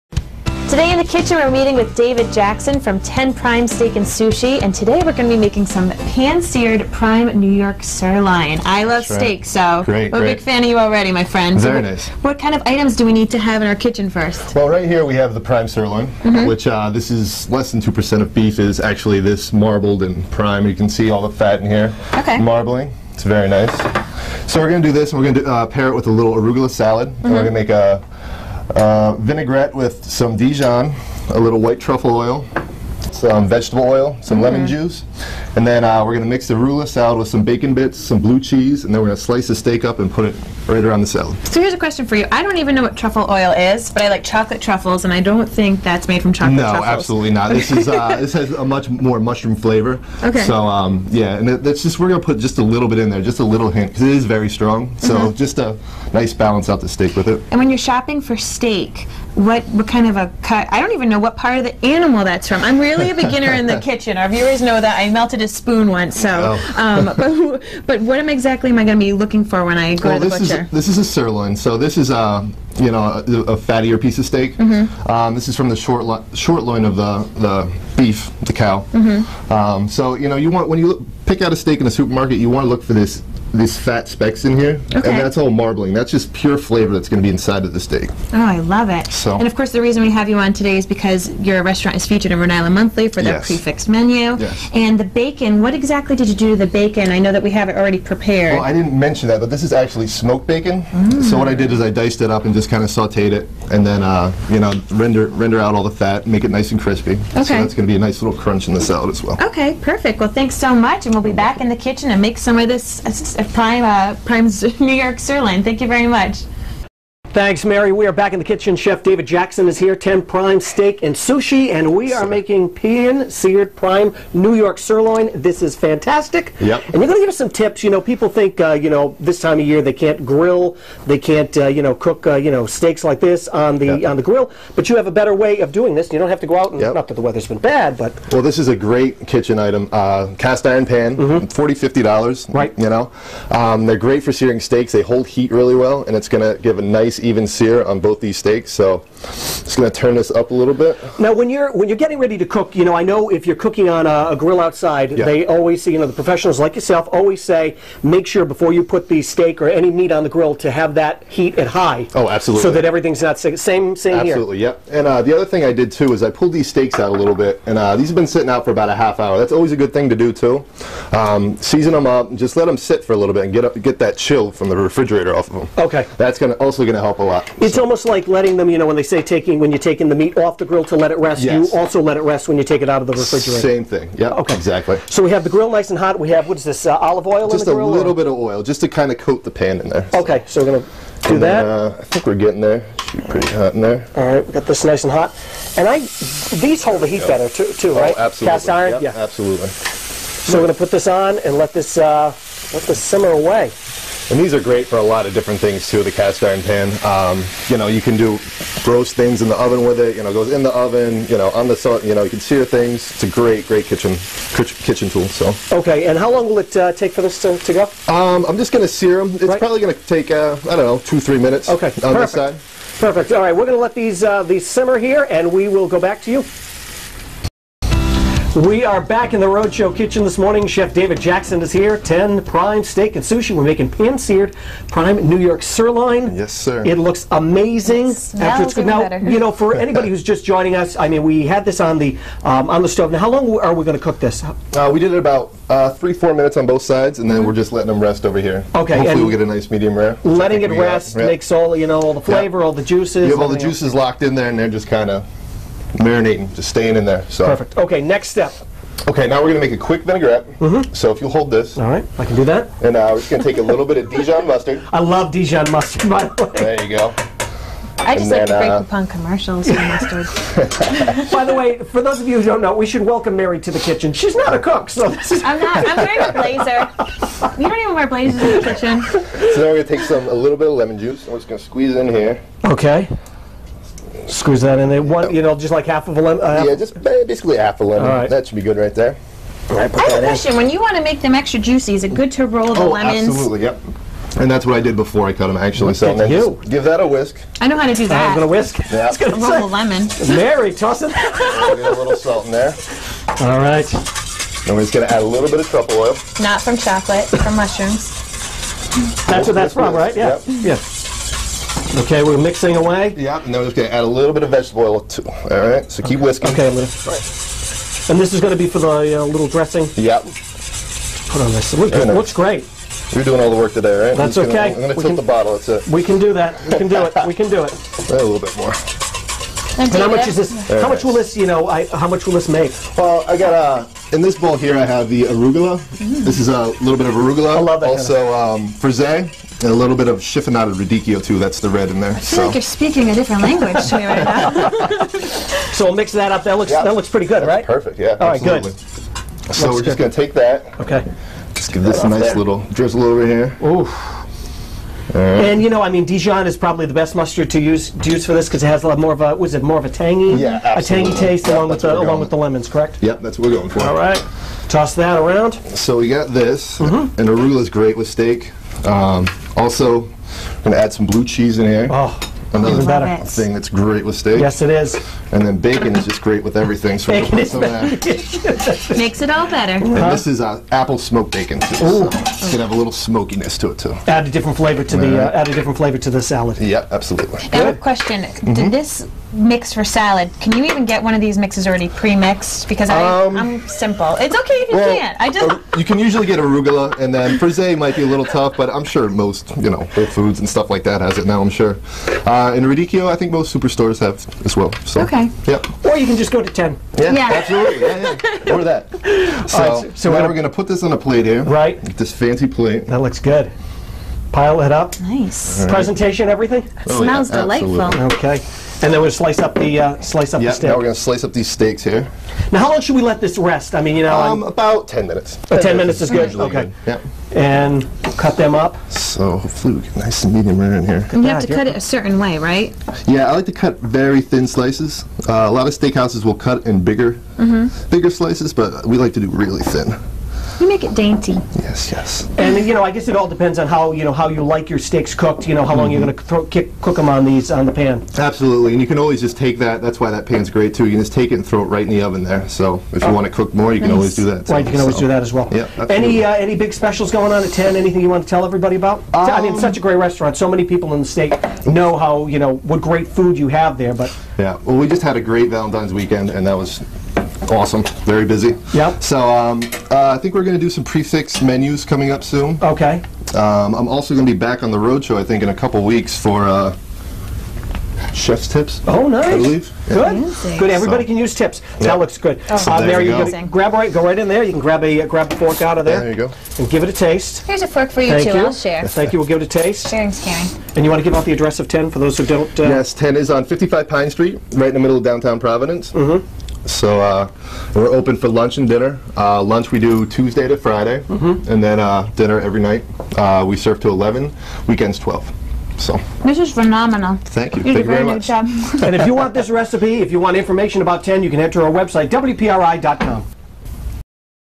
Cooking: Pan Seared Prime New York Sirloin
We're making Pan Seared Prime New York Sirloin, Baby Arugula Salad with Blue Cheese, Smoked Bacon and Dijon Truffle Vinaigrette with Chef David Jackson of Ten Prime Steak & Sushi.